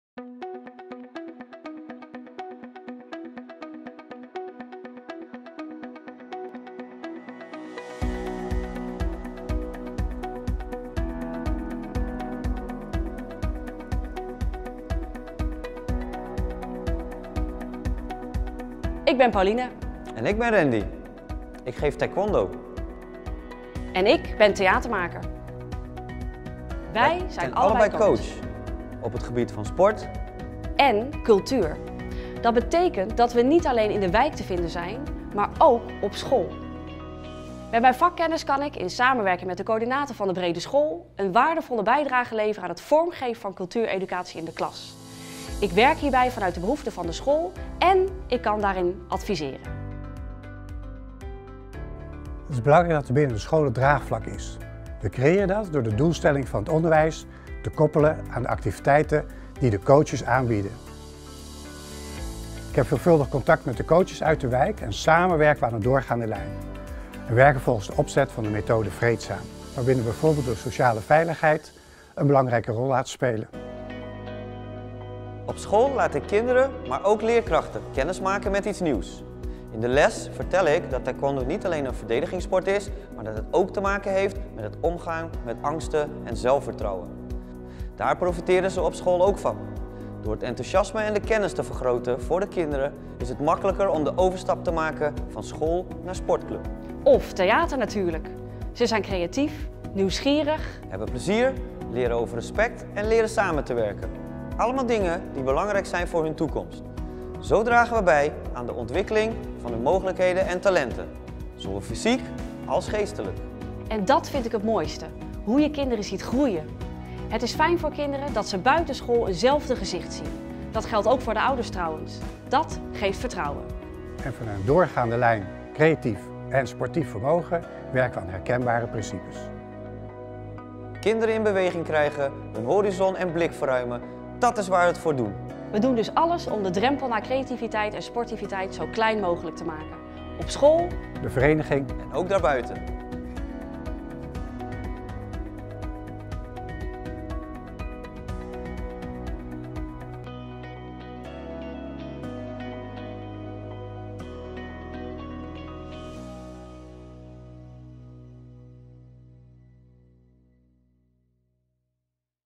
Combinatiefunctionaris in Utrecht
Wil jij weten wat een combinatiefunctionaris doet in Utrecht?
In deze film leggen we uit hoe combinatiefunctionarissen zorgen voor verbinding tussen onderwijs en sport of onderwijs en cultuur.